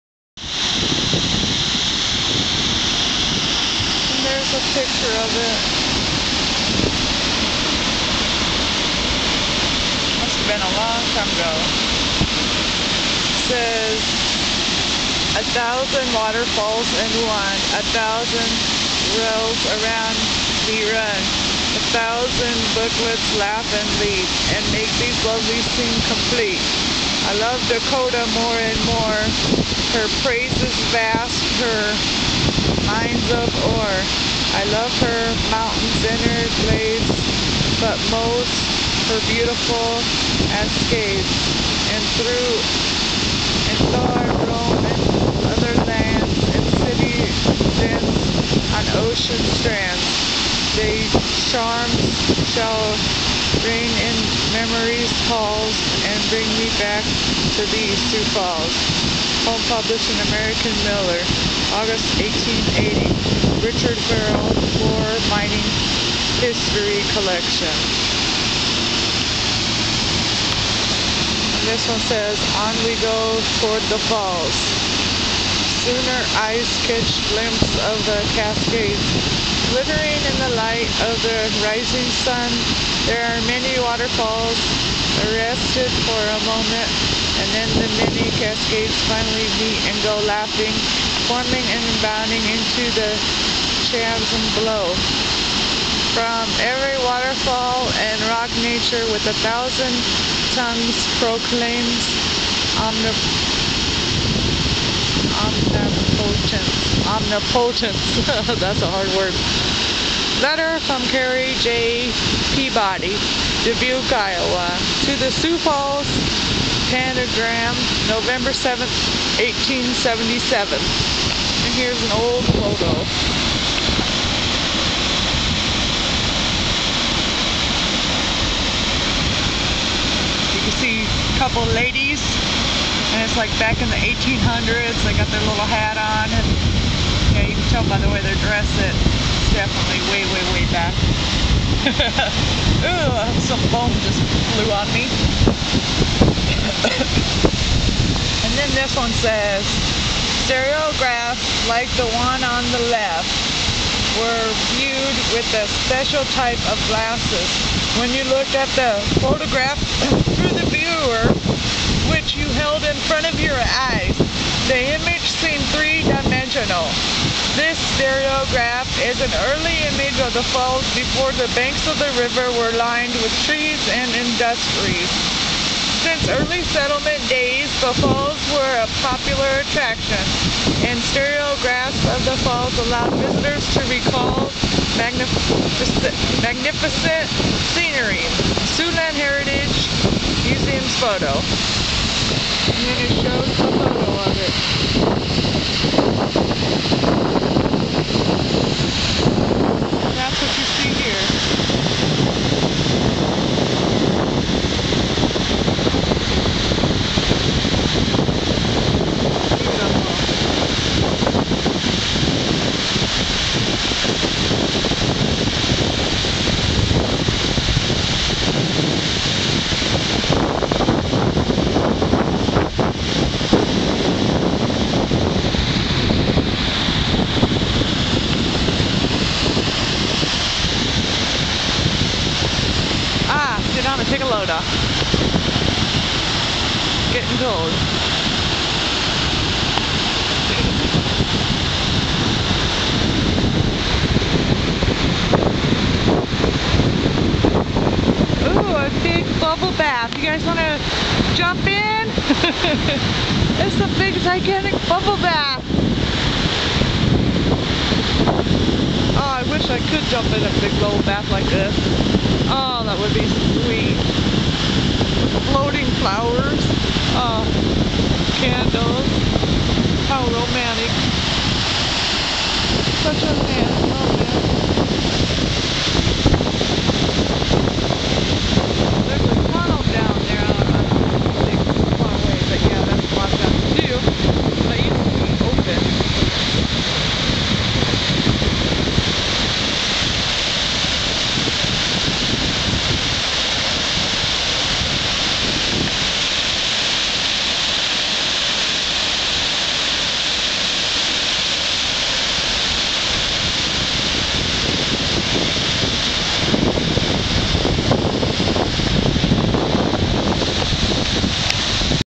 The Lure Part # 2
wanna see my hotpics?

Sioux land Heritage Museums Photos and poems from back in the late 1800's.The Falls Raging River water falls come over the banks at high speeds per second.